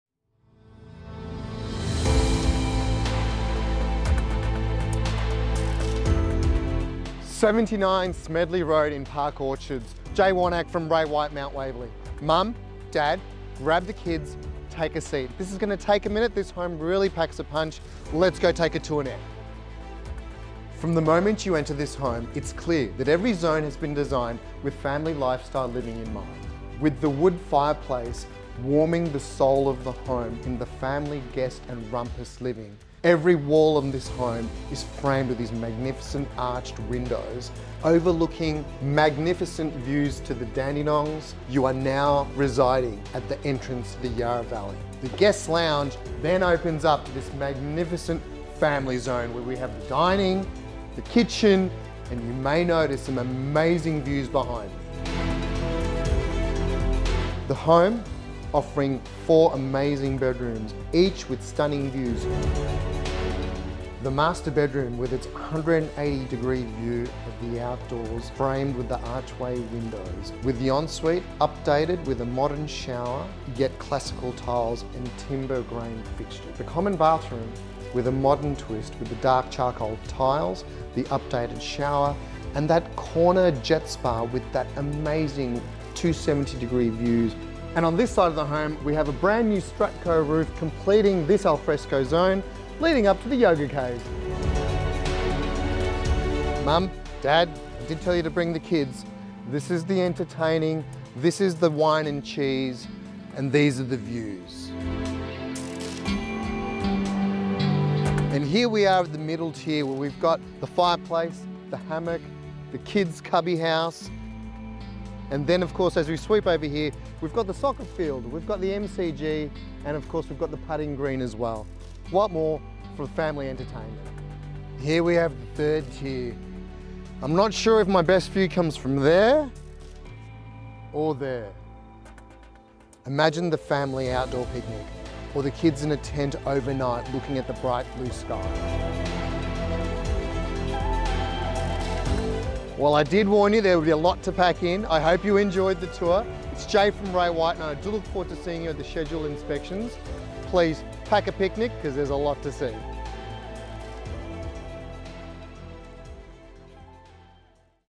79 Smedley Road, Park Orchards
PICTURESQUE ENTERTAINER'S SANCTUARY

An oasis of calming tranquillity that frames verdant vistas of the outside, this secluded lifestyle retreat relishes contemporary attributes and family convenience upon a beautifully terraced acre (approx.) allotment that marries indoor and outdoor entertaining. Delivering an incredible sense of being at one with nature, stunning arched windows draw your gaze out into the garden and infuse the interior with natural light whilst complementing the various living zones. Acting as a welcome introduction, the formal lounge room faces towards an open fireplace before flowing through to the indulgent kitchen, dining and family room that flaunts stone benches, quality appliances, soft close drawers plus a breakfast bench. Stretching out on either side, the home's various entertaining zones includes a northerly oriented alfresco zone that's overlooked by a yoga studio, while the sun blessed entertaining deck invites you down to the child-friendly lawn, cubby house and firepit area. Nestled together along the hallway, the four bedrooms embrace picturesque garden aspects and include a huge master suite with parent's sitting zone, private outdoor area, built-in-robe and dual vanity ensuite. Further complemented by a dual vanity spa bathroom, separate toilet, laundry, ducted heating, split system air conditioning, keyless entry, shade sail guest parking plus a double garage and extra parking beside the circular driveway. As for the location, you're zoned to Norwood Secondary and close to Park Orchards Primary, Whitefriars College, Yarra Valley Grammar, Ringwood North shops, Eastland Shopping Centre plus EastLink/Eastern Freeway.